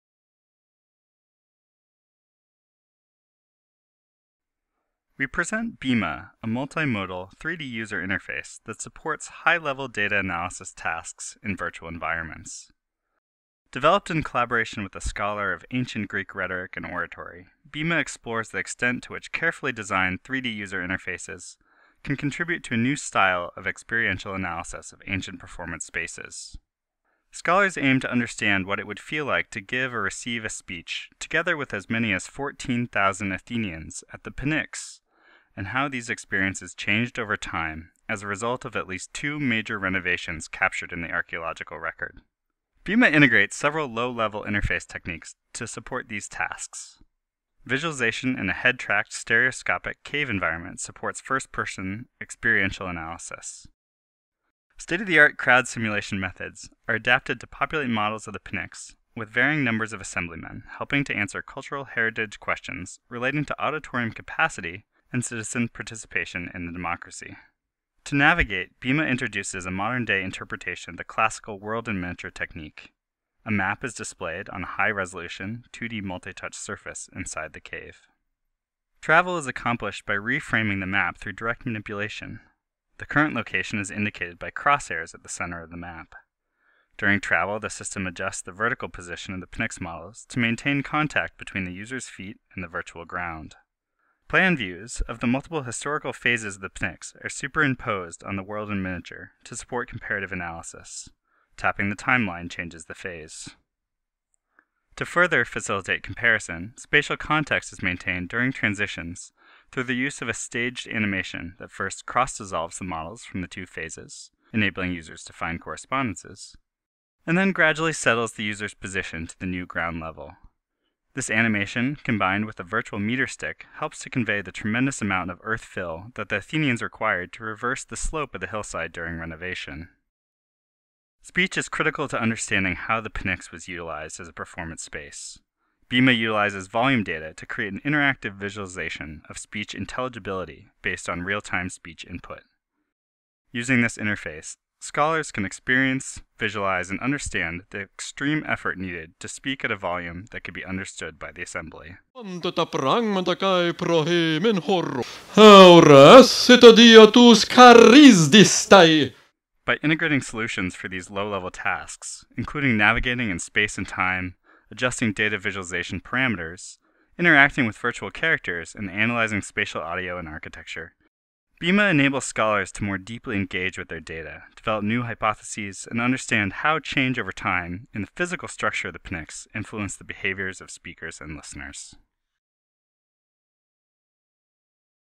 Bema: Experiential Analysis of Assemblies at the Pnyx in Ancient Greece
We present Bema, a multimodal user interface that enables scholars of Greek rhetoric and oratory to perform virtual reality studies of ancient political assemblies at the hill of the Pnyx. Named after the flat stone speakers’ platform utilized at the Pnyx, the Bema interface supports the high-level task of gaining a nuanced understanding of what it would feel like to give or receive a speech together with as many as 14,000 Athenian citizens and, further, how this experience must have changed as a result of at least two massive renovations captured in the archaeological record. Bema integrates solutions for several low-level interaction tasks, including navigating in virtual space and time, adjusting data visualization parameters, interact- ing with virtual characters, and analyzing spatial audio and archi- tecture. Navigation is accomplished through a World-in-Miniature technique, re-conceived to support multi-touch input within a 4- wall Cave environment. Six degree-of-freedom head tracking and a sound level meter are used to analyze speeches delivered by users. Comparative analysis of different historical phases and assembly sizes is facilitated by the use of crowd simulation to generate realis- tic spatial arrangements for the assemblymen and staged animated transitions that preserve context while comparing two or more sce- narios. An evaluation with our team’s scholar of ancient Greek rhetoric and oratory provides support for the most important design decisions and affirms the value of this user interface for experiential analysis

   Title - Bema: A Multimodal Interface for Expert Experiential Analysis of Political Assemblies at the Pnyx in Ancient Greece
   Authors - Kyungyoon Kim, Bret Jackson, Ioannis Karamouzas, Moses Adeagbo, Stephen J. Guy, Richard Graff, Daniel F. Keefe